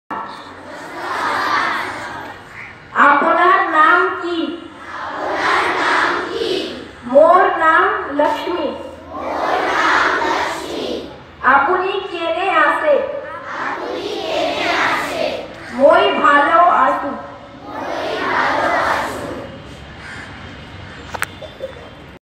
Assamese 21Nov
(i) Assamese
(ii) Agarpara Swami Vivekananda Academy
(iii) 3 B.T. Road, Agarpara, North 24 Paraganas, Kolkata-700058
(iv) West Bengal
(v) The five sentences partening to the Assamese language read out by the students and teachers. One of the senior student of our school lead the total activities.